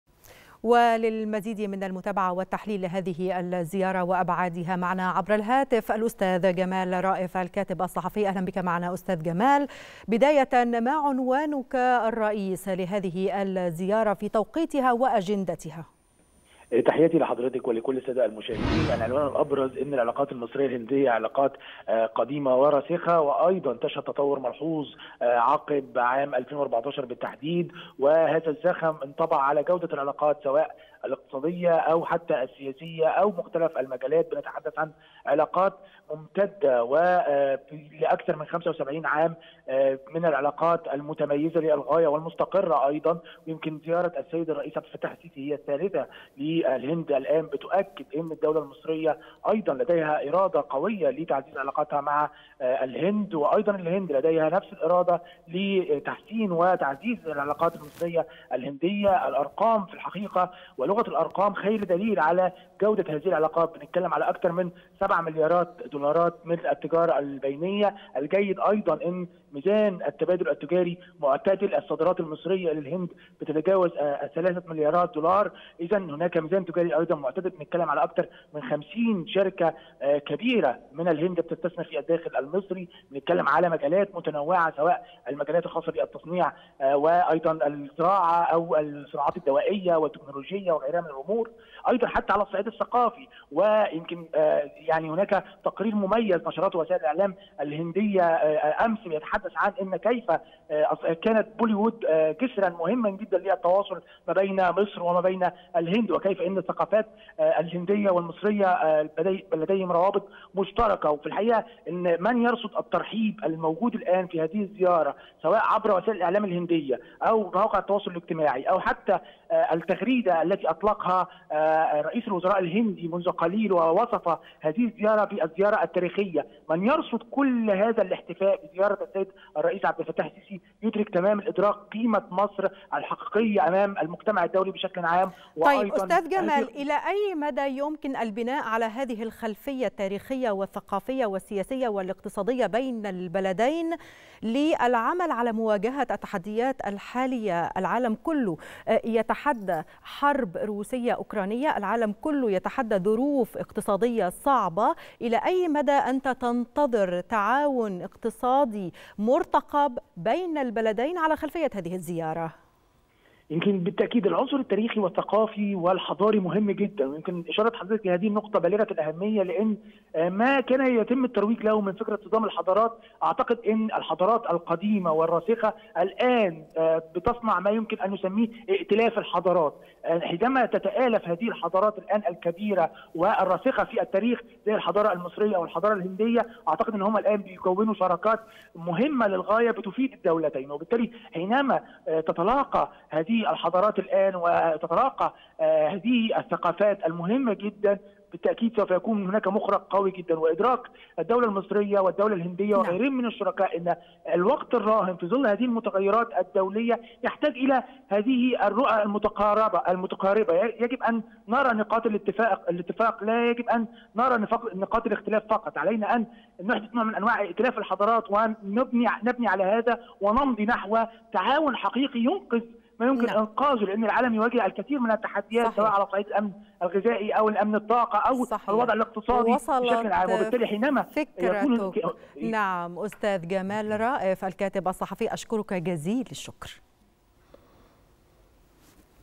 الصحفي جمال رائف.. يستعرض أهمية زيارة الرئيس السيسي إلى الهند كضيف شرف في احتفالات يوم الجمهورية
نشرة التاسعة | الصحفي جمال رائف.. يستعرض أهمية زيارة الرئيس السيسي إلى الهند كضيف شرف في احتفالات يوم الجمهورية